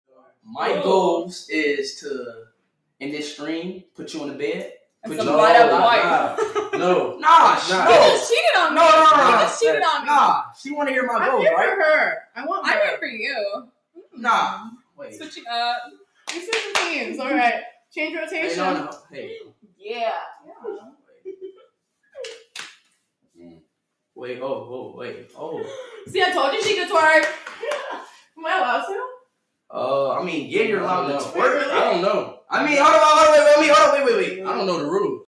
iShowSpeed X Dad Get Bricked When These ig Models Did This
iShowSpeed & His Dad 2 Baddies give eachother a lap dance